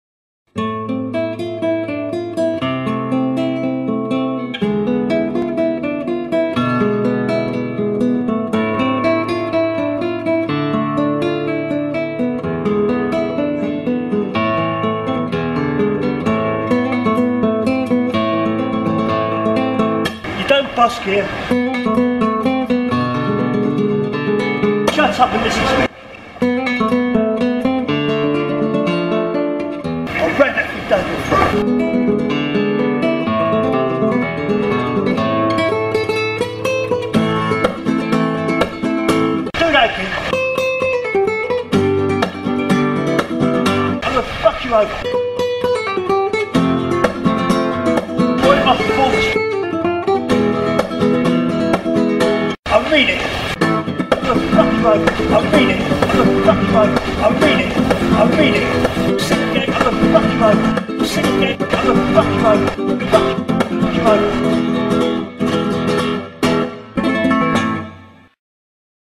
Baths abusive busker.
I was a little outraged by Mr Millhouses actions toward another disabled busker. I thought i'd "make a punishment fit the crime..."